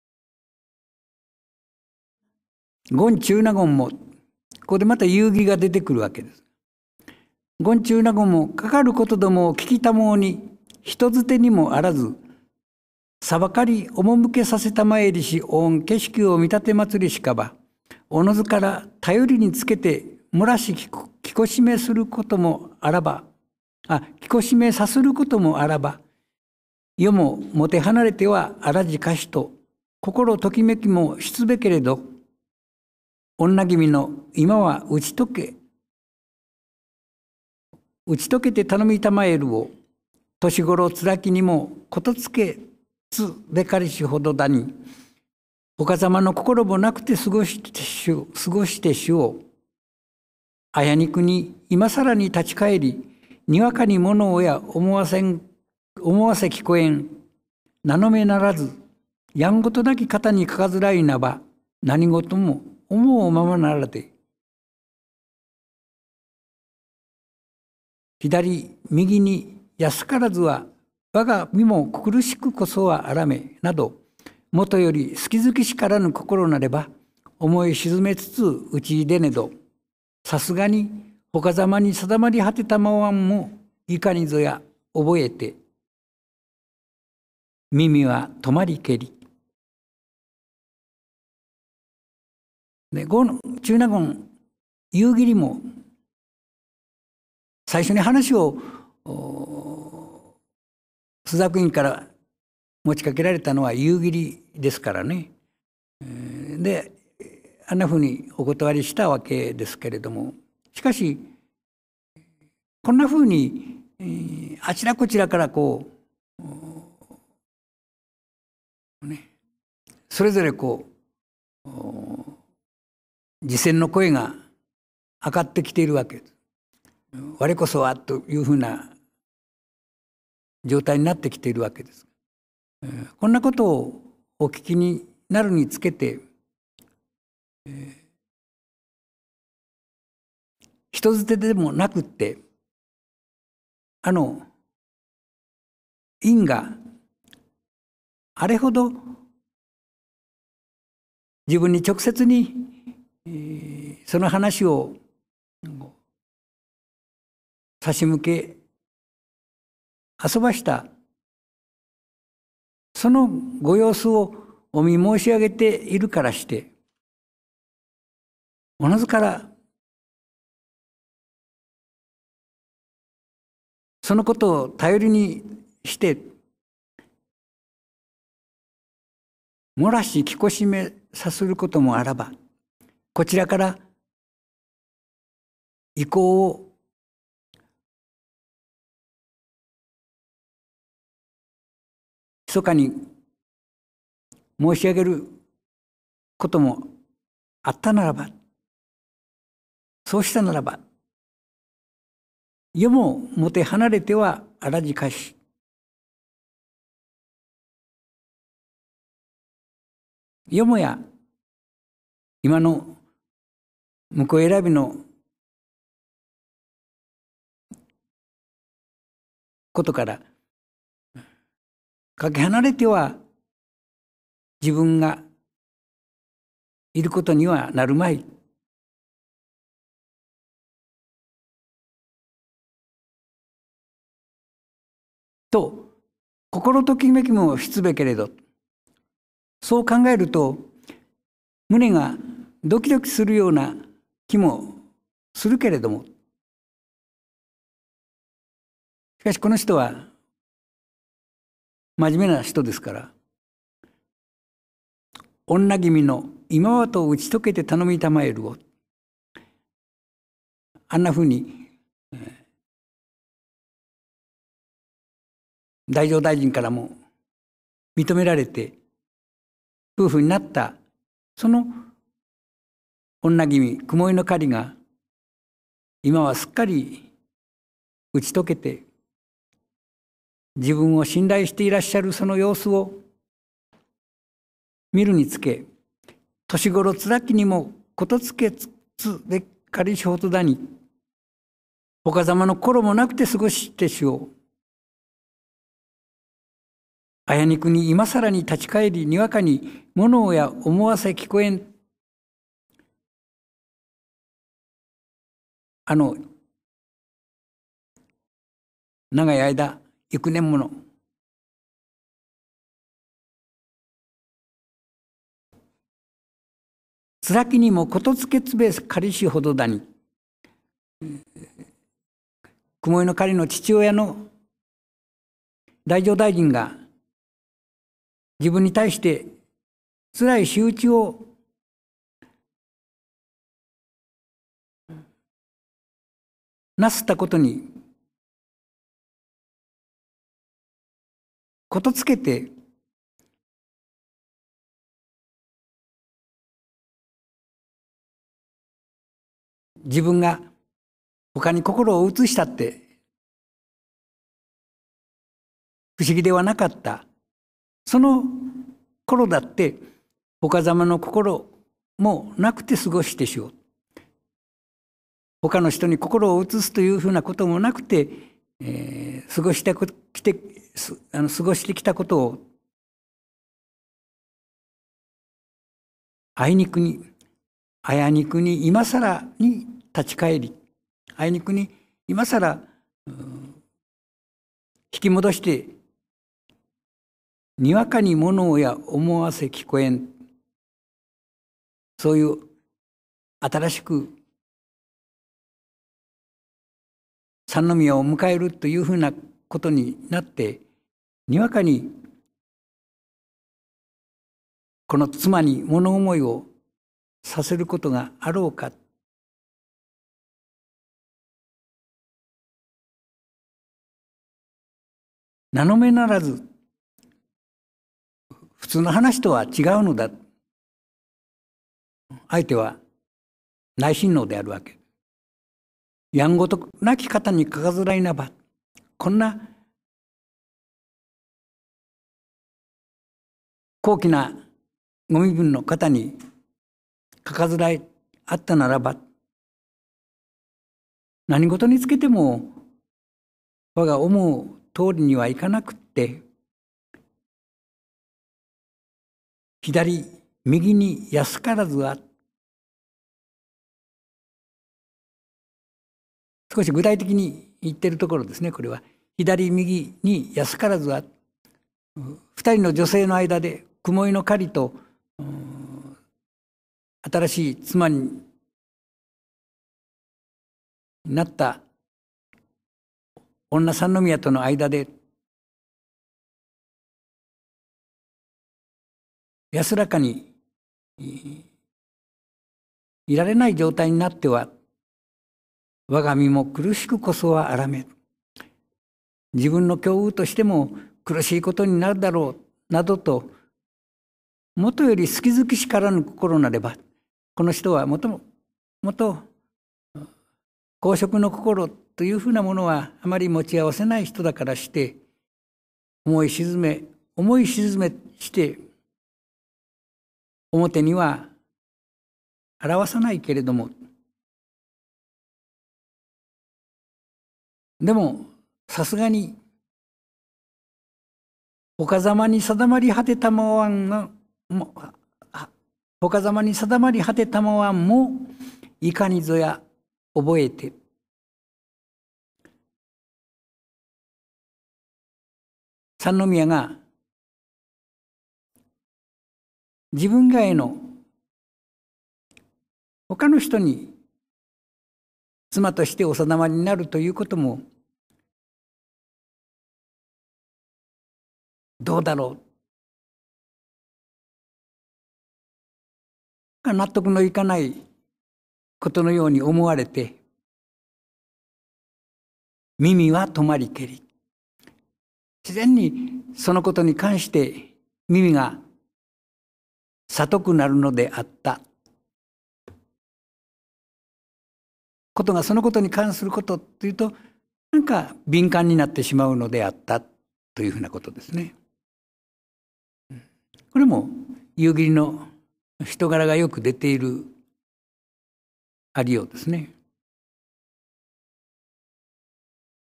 10.権中納言も、かかることどもを聞きたまふに、
源氏物語全講会 第132回 「若菜上」より その2

講師　岡野弘彦（國學院大學名誉教授）
講座名　平成21年秋期講座